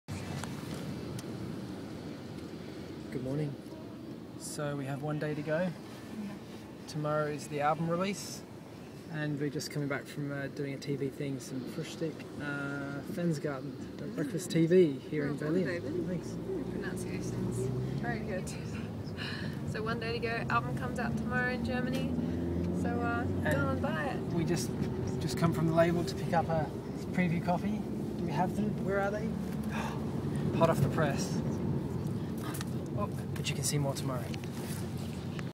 1 day to go!!!! TV & Frühstück
So tomorrows the album release here in Germany! We're doing some more interviews today to spread the word... We had a rather early morning start playing on the breakfast show FRÜHSTÜCKSFERNSEHEN.